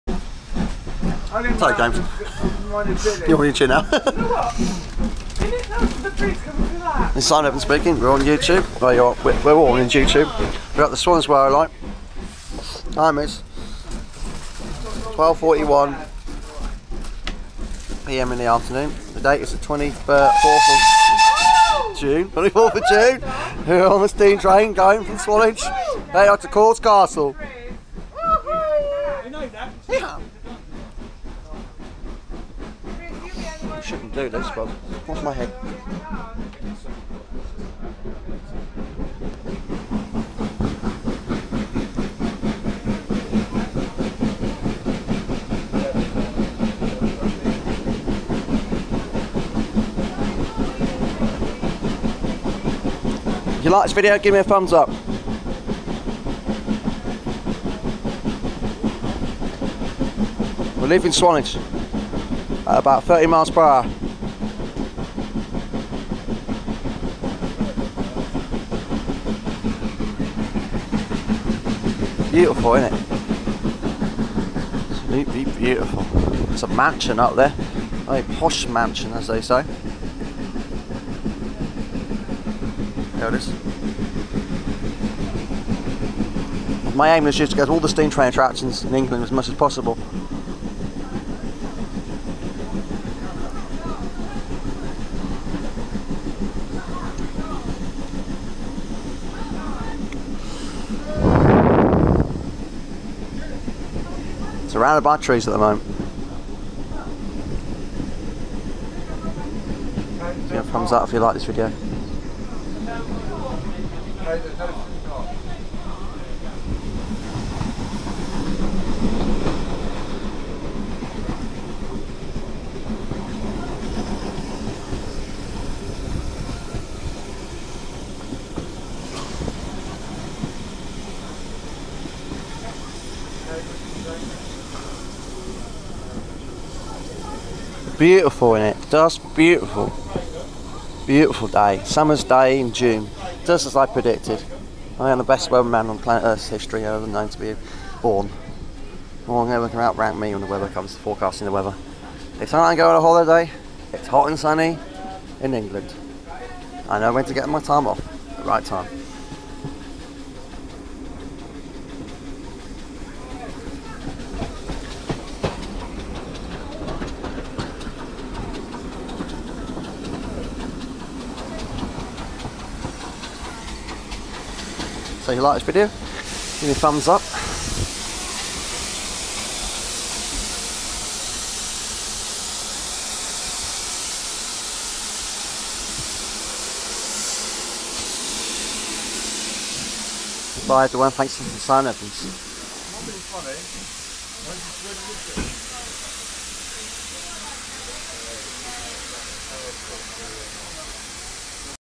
VIDEO OF A STEAM TRAIN TRIP.
On are way up to Core castle on the steam train.